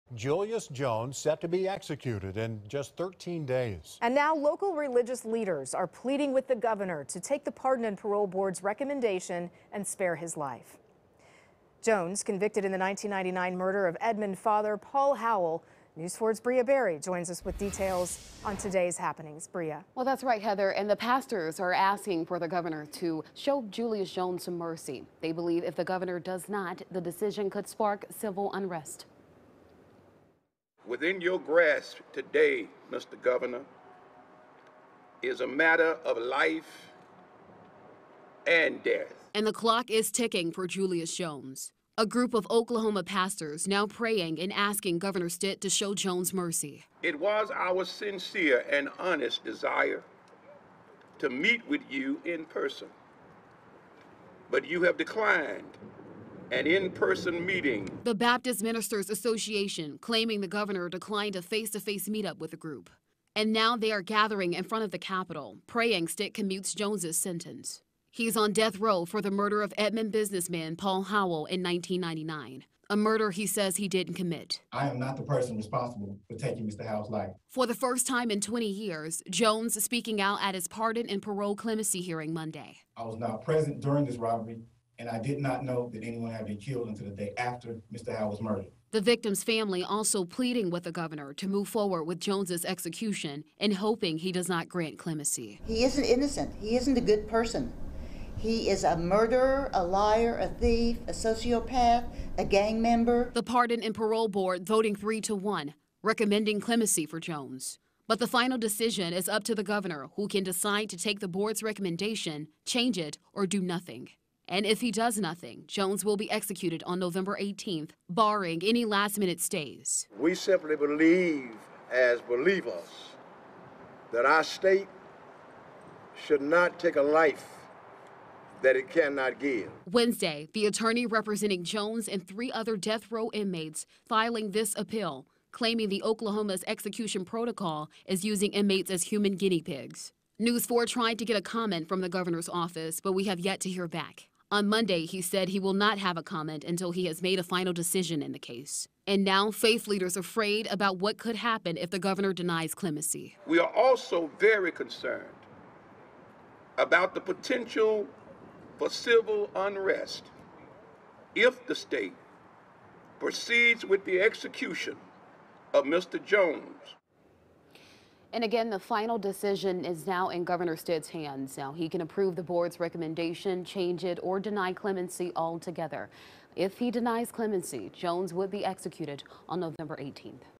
Oklahoma pastors hold prayer vigil - afraid of what could happen if Julius Jones is executed
A group of Oklahoma pastors is praying that Governor Stitt will show mercy to death row inmate Julius Jones.